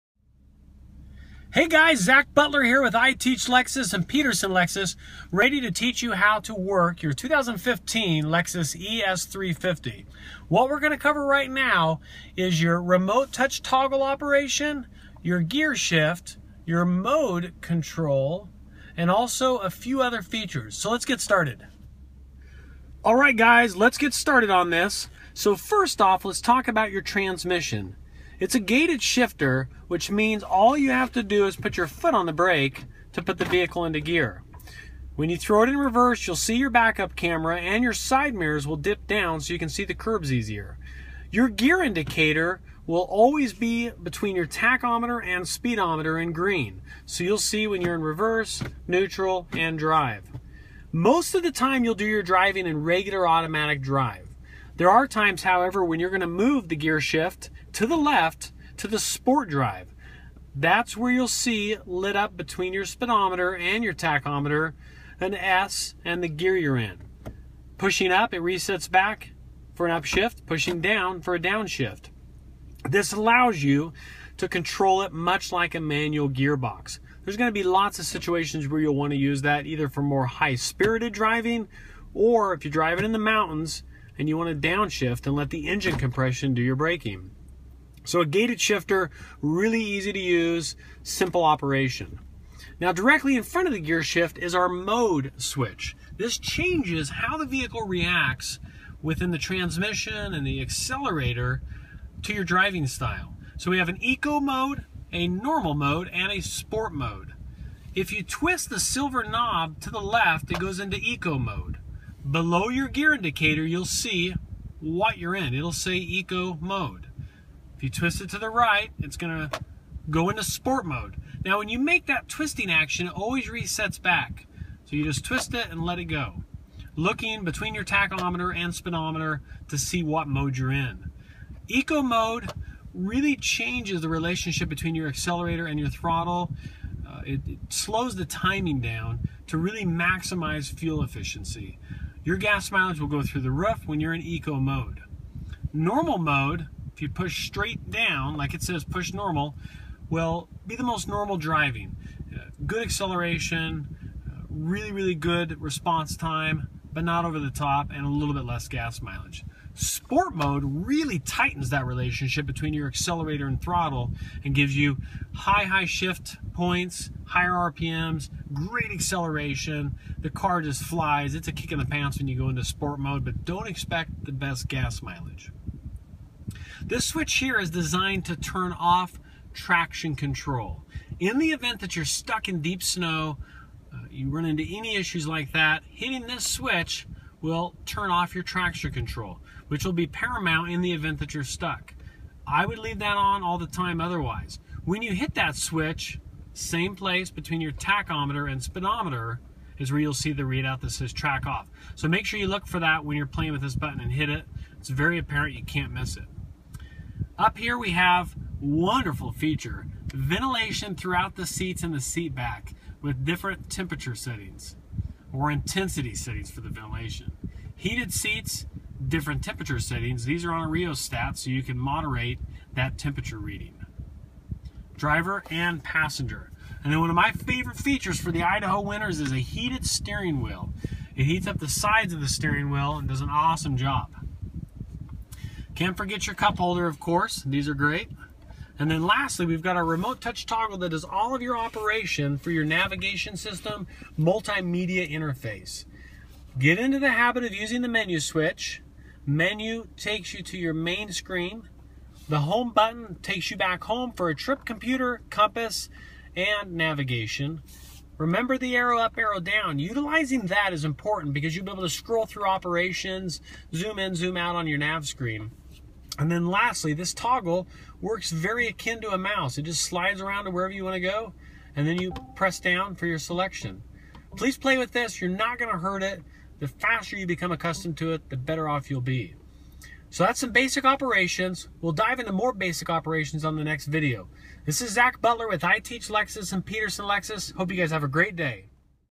iTeachLexus 2015 ES350 Remote Touch, Transmission, Drive Mode Select How To
Zach Butler with Peterson Lexus and iTeachLexus covers how to operate your ES350 Transmission, Drive Select Mode, and more!

For more help and instruction with your new Lexus make sure